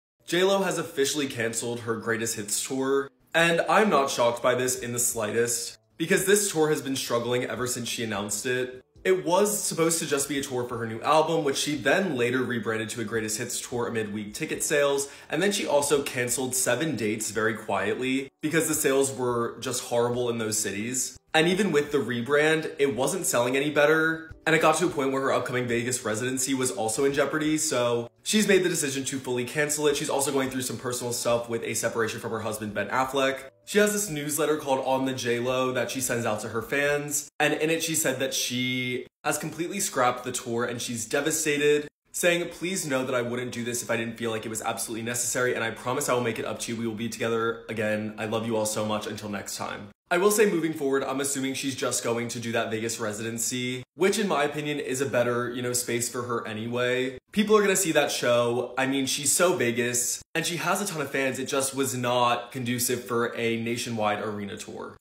Why Jennifer Lopez has CANCELLED her tour?? tiktok holdensmith962
Support Pop Rumors:
If you enjoy the videos and want to contribute to the channel's growth, consider making a donation through